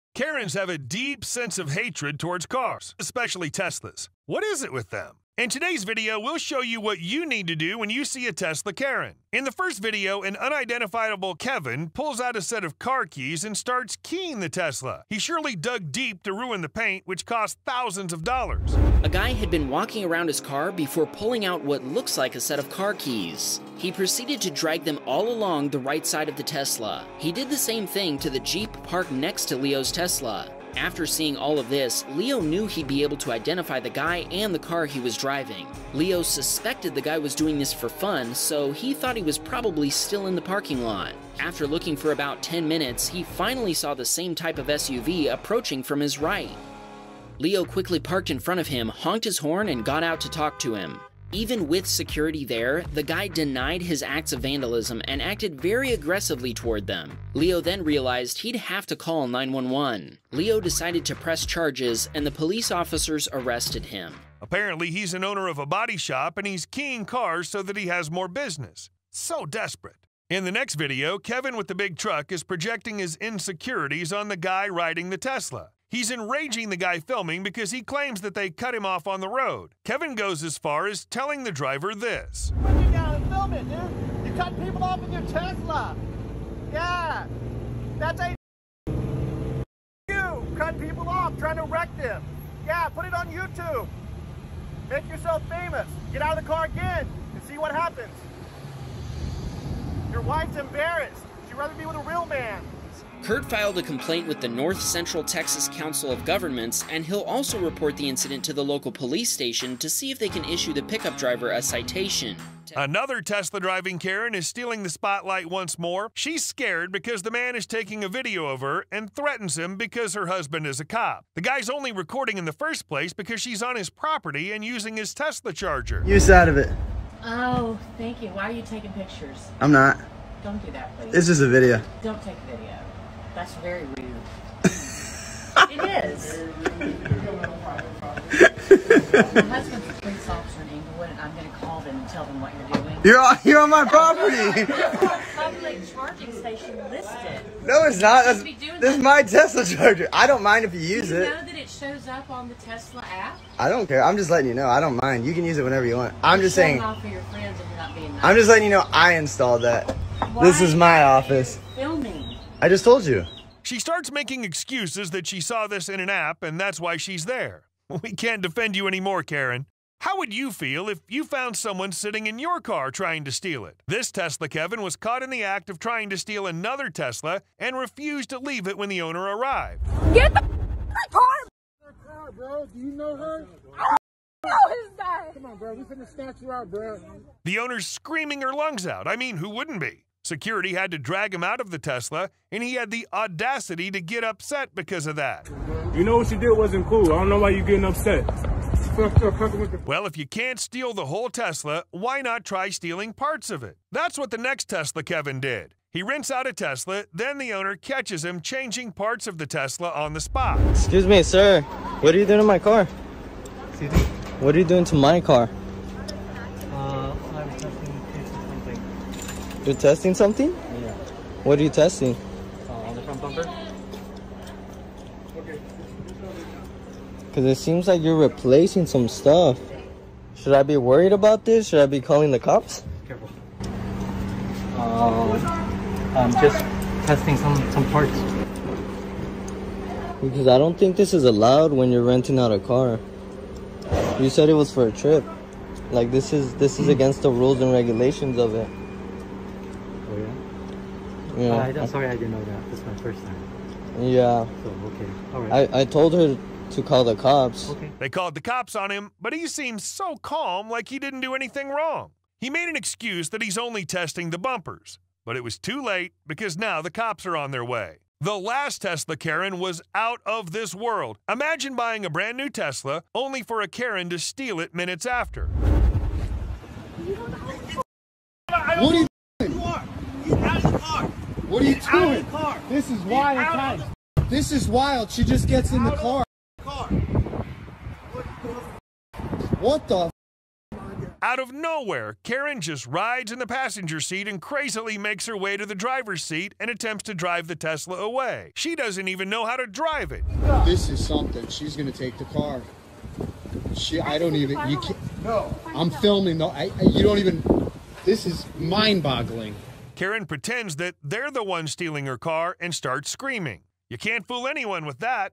KAREN Tries Stealing My Tesla... (ARRESTED)
KAREN Tries Stealing My Tesla... (INSTANT REGRET)

Clips are from WhamBam TeslaCam and Tiktok.

If your clip is in this video and you want it to be removed, please e-mail . Thank you for your understanding.

Some tags that apply to this video are: TeslaKarens KarenStealingTesla​